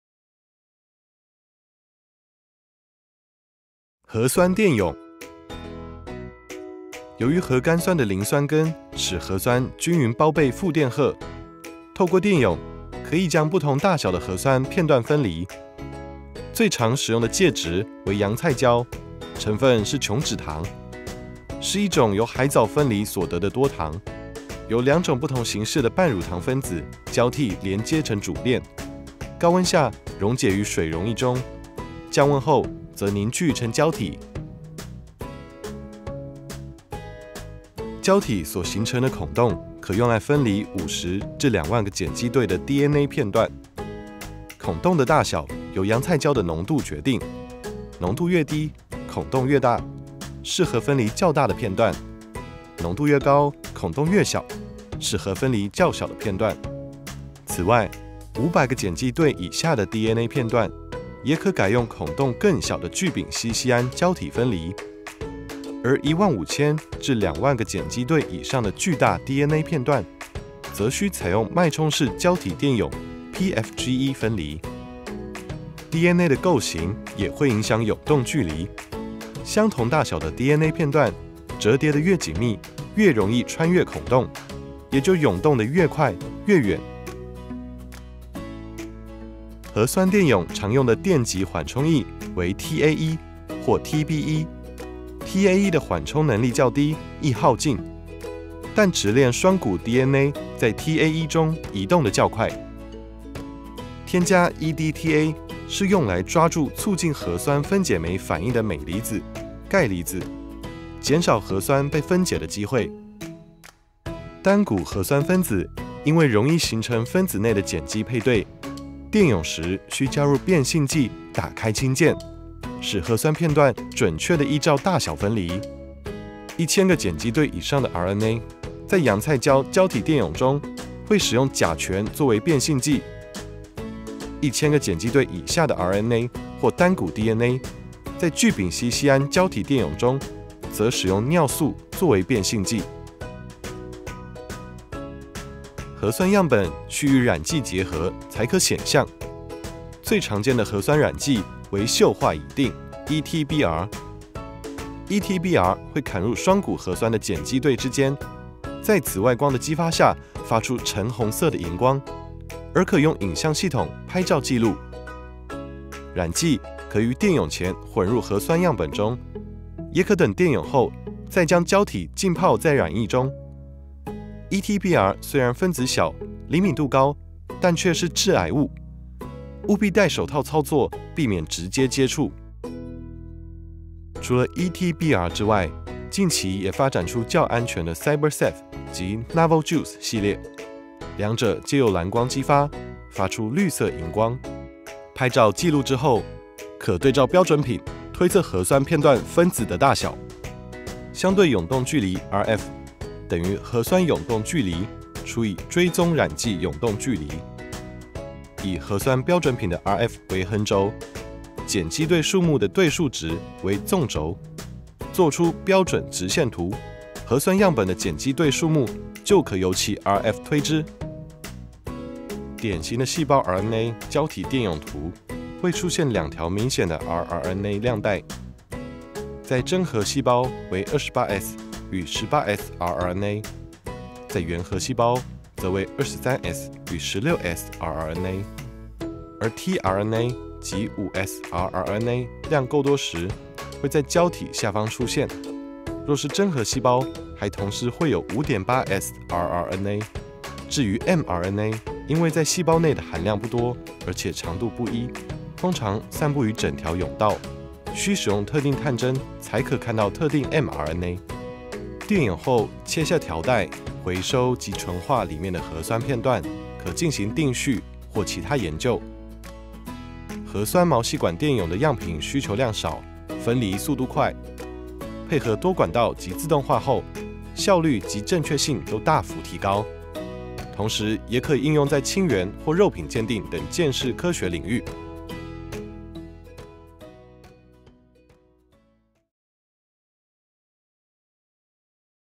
(5-1) 核酸電泳_原理 (動畫)
臺大開放式課程
課程名稱：生化實驗技術：原理 (動畫) 與操作示範
授課教師：生物技術研究中心 生化實驗教學資源工作室 王惠鈞 等